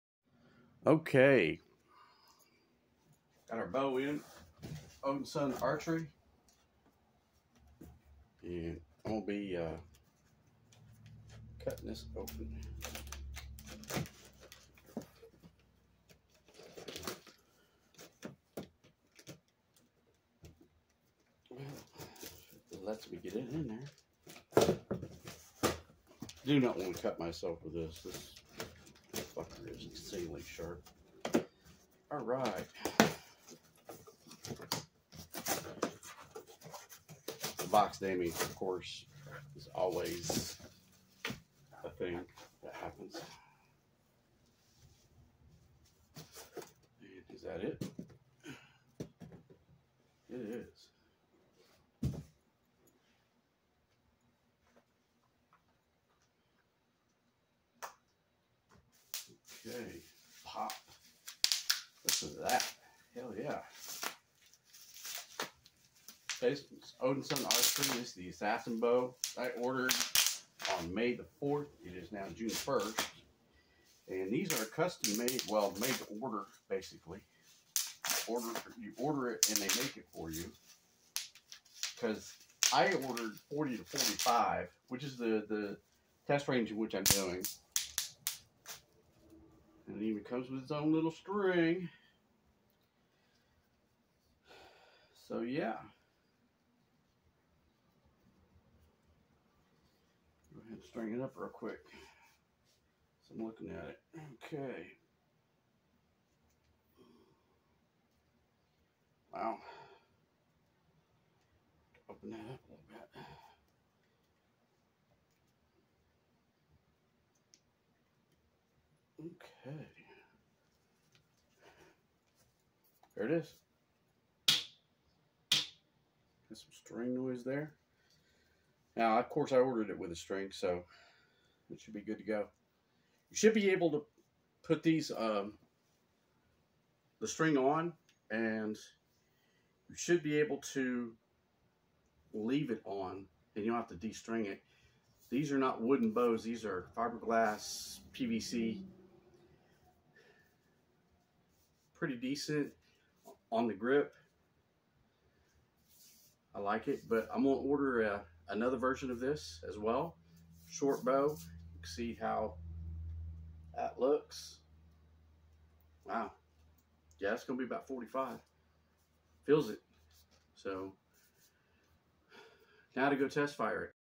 Jim's Budget Archery - Odinson Creations, Assassins Bow Unboxing! archery traditionalbow 🎯
This bow seems very solid! I can't wait to test it out.  It will be 90 degrees today! so I will get up early in the coming days to avoid the heat wave we are currently going through.